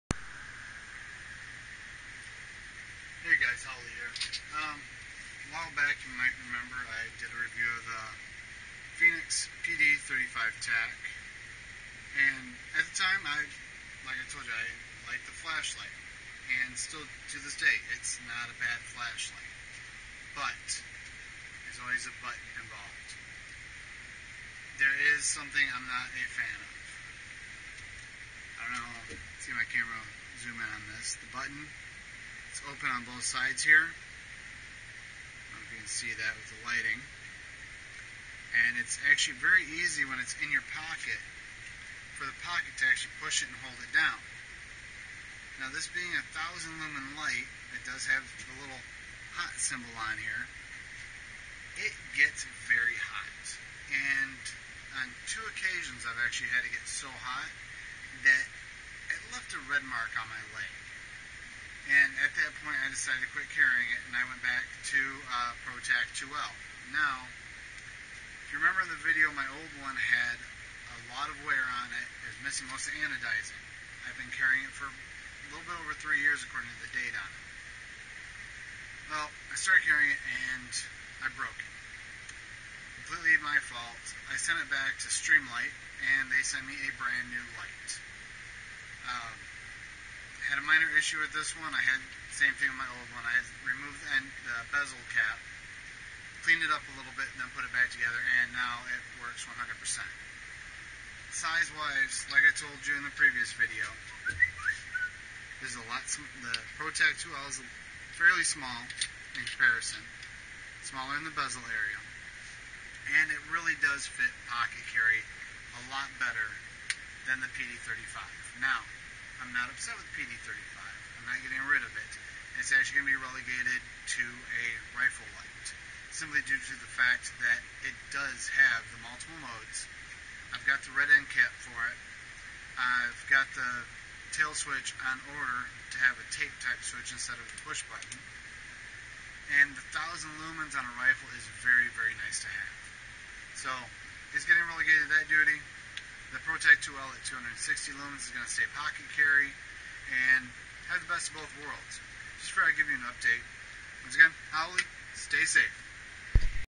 Fenix PD35 TAC Followup
Well, here is my follow-up on the PD35.  Solid light, and can't say I don't like it, just not the right light for pocket carry.  I have gone back to my Streamlight Protac 2L for everyday carry and moved my PD35 to AR duty.